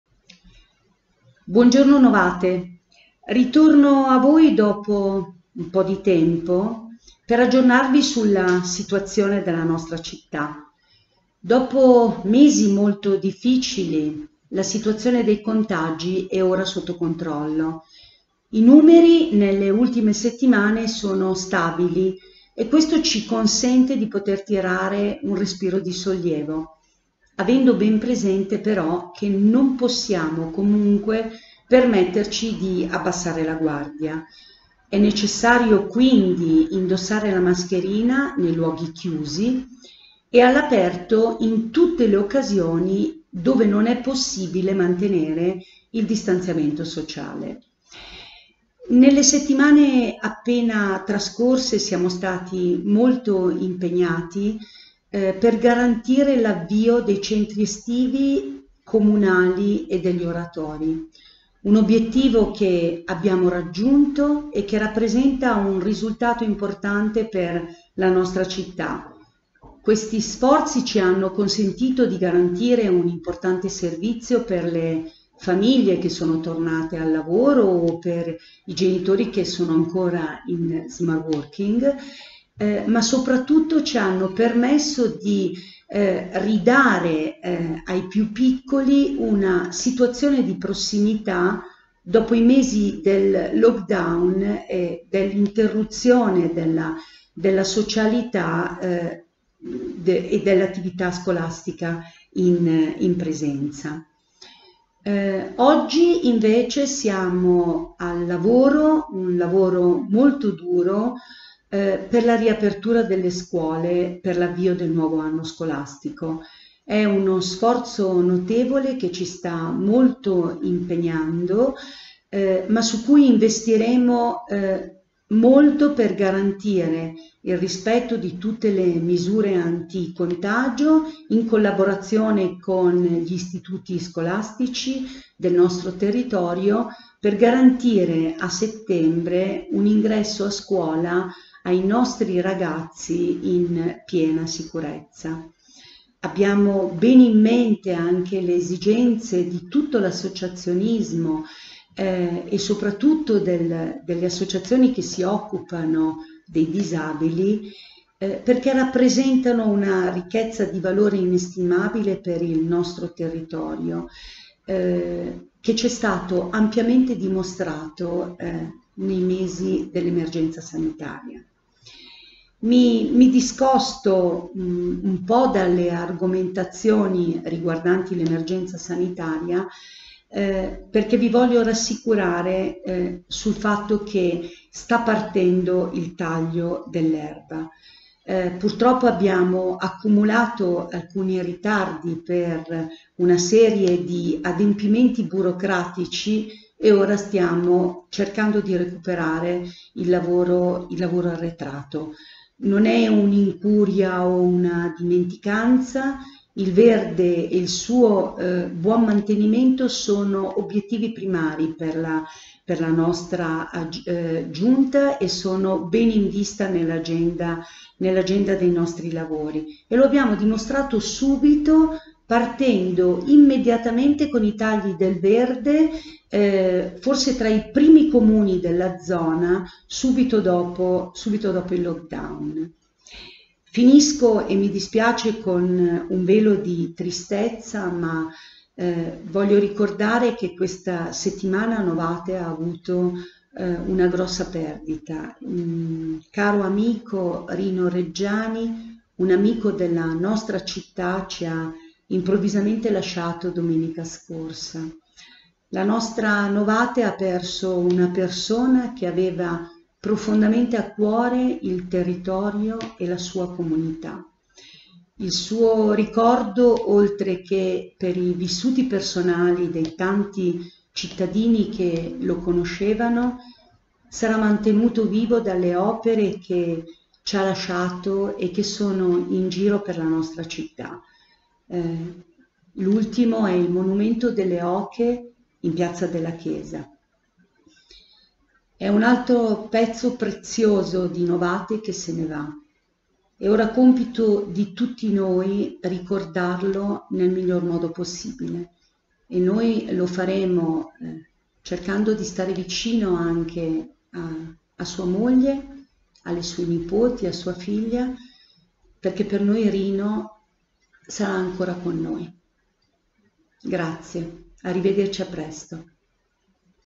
Messaggio del Sindaco ai novatesi
Il messaggio del Sindaco, Daniela Maldini, alla città di Novate Milanese